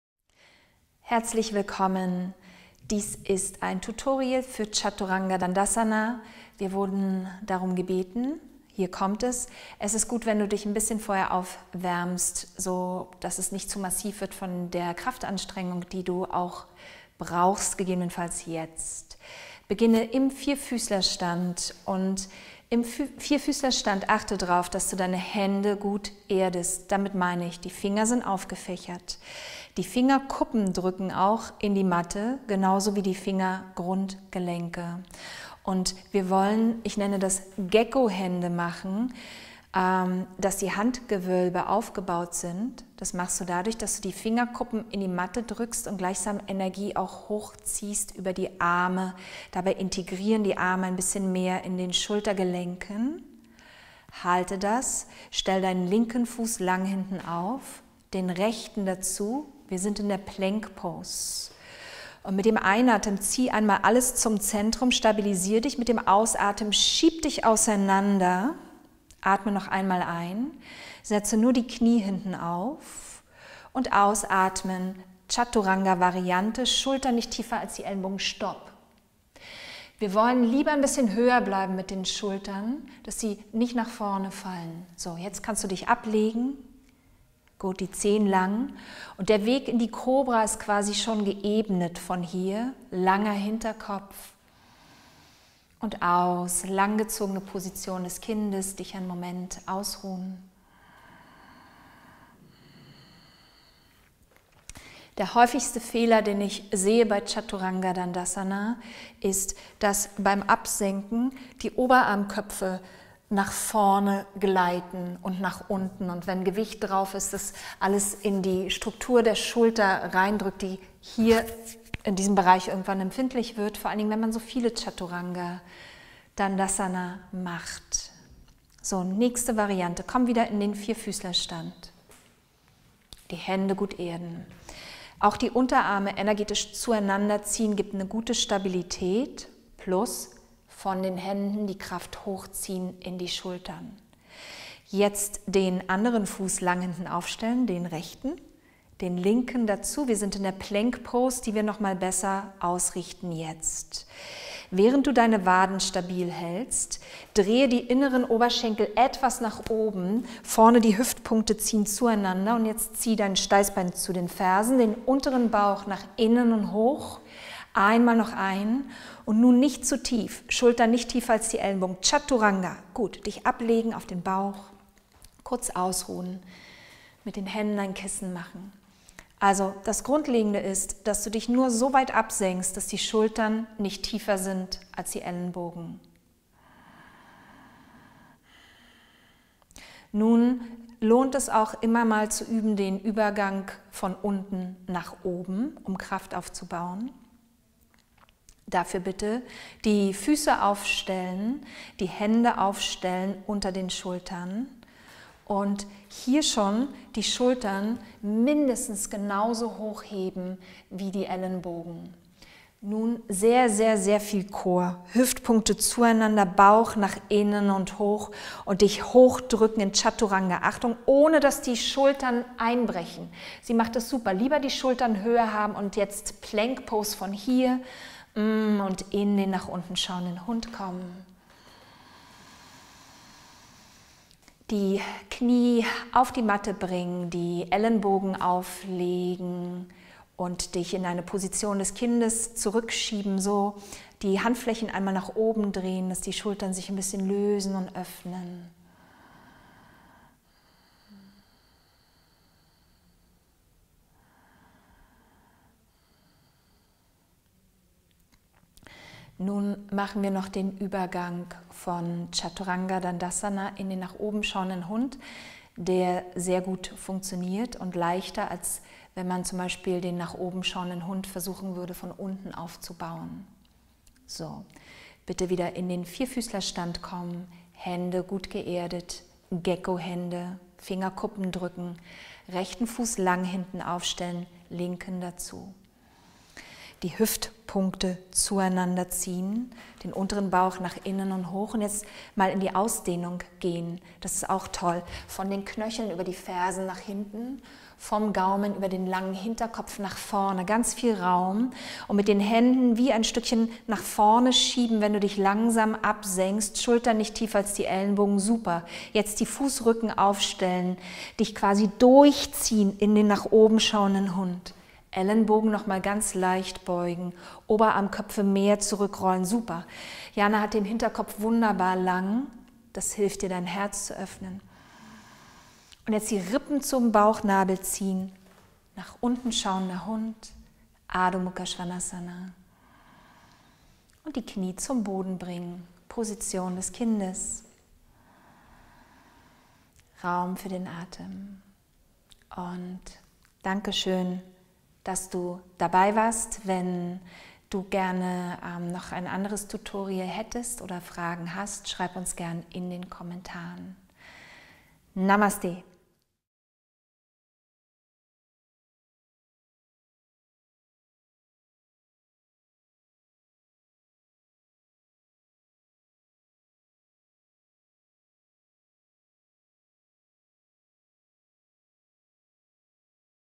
Yoga - Tutorial: CHATURANGA DANDASANA im Anusara®Yoga mit Veronika Freitag
In diesem Tutorial widmen wir uns Chaturange Dandasana und zeigen dir,  worauf es bei der Ausrichtung ankommt.  Viel Freude.  Wenn du Fragen hast, oder weitere Stellungen besprochen haben möchtest, lass es uns gerne in den Kommentaren wissen.
Für Spenden für das JYOTI/Veronika Freitag, sind wir sehr dankbar.
an Kontoinhaberin: Veronika Freitag
DE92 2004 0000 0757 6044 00
Commerzbank Hamburg
Verwendungszweck: Spende